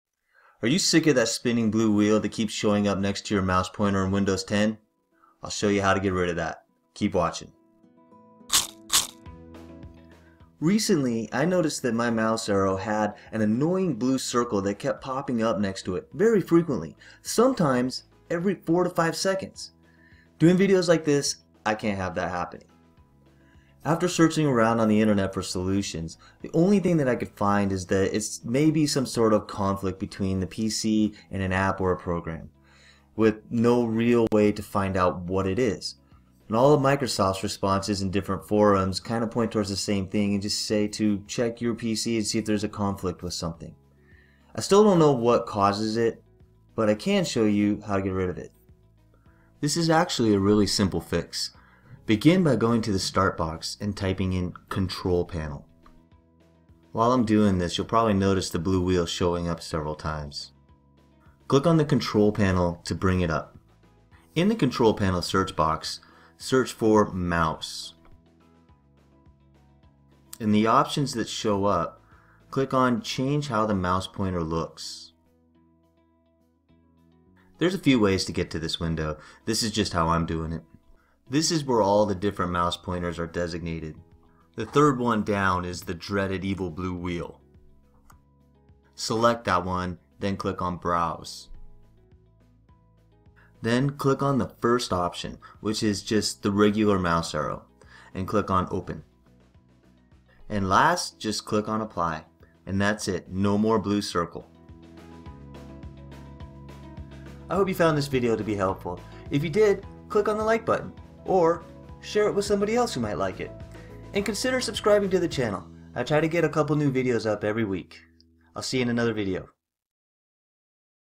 Windows Spinning Blue Circle Fix
Get rid of the blue spinning wheel that keeps showing up next to your mouse pointer in Windows 10!

*Update to Address Some of the Comments*
This video does not diagnose the reason behind the blue circle. It just shows you how to make it go away, which is what most of you want. A good majority of the time, the cause behind the circle is not affecting performance, but you just don't want to keep seeing it pop up.

For those who are trying go deep and fix the root of the problem, this isn't the video for you. I state this right in the beginning of the video.

I did a bunch of other 'fixes' before just doing this. Uninstalled apps/programs, changed startup apps, etc. Nothing worked. At this point, I don't really care what's making it happen. My system runs just fine. I just needed the evil blue circle to be gone!

Please note that this doesn’t fix the underlying problem, but it definitely gets the annoying circle out of your face!

Check out my best selling Google Cloud Productivity course on Udemy to learn Docs, Sheets, Slides and Forms